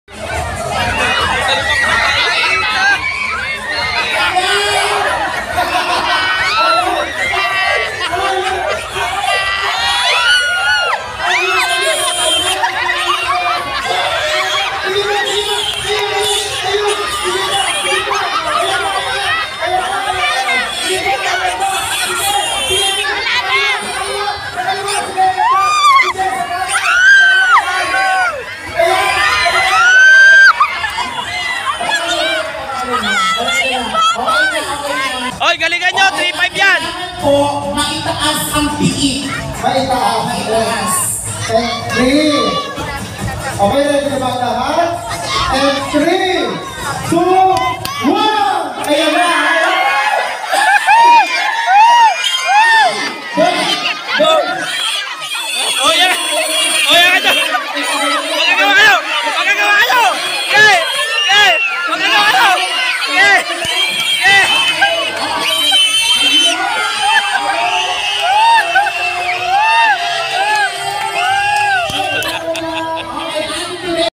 UNAHANG MAKAHULI NG BABOY, GRABE NAGING PANG UFC.
HULIHIN ANG BABOY, NAGING PANG UFC ANG LARO.